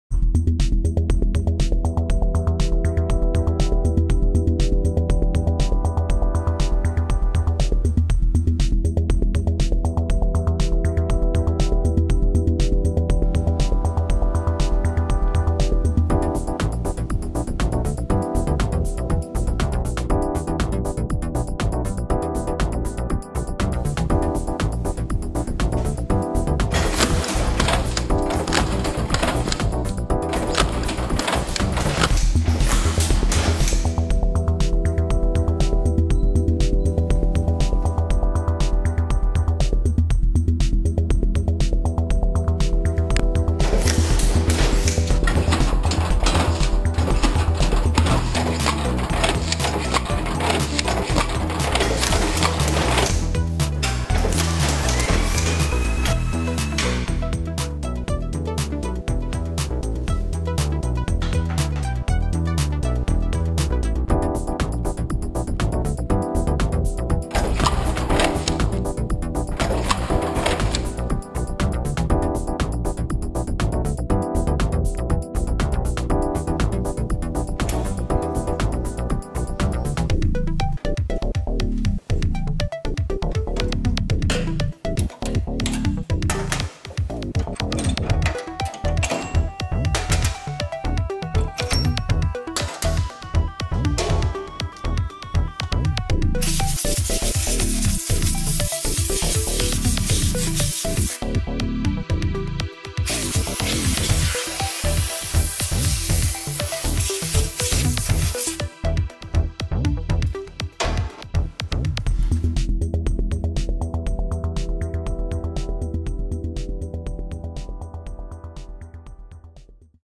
Mosca ROMP5 (Version Français)
Les systèmes de cerclage Mosca sont reconnus pour leur performance supérieure dans une multitude d'applications variées. La ROMP5 est un système de cerclage dont les cycles sont déterminés par l'opérateur ; elle comporte un moteur DC sans brosse à enclenchement direct. L'ensemble de ses attributs, sa vitesse et sa fiabilité font qu'elle est la solution de premier choix pour les usagers orientés productivité désirant retirer la meilleure performance de leurs procédés de production.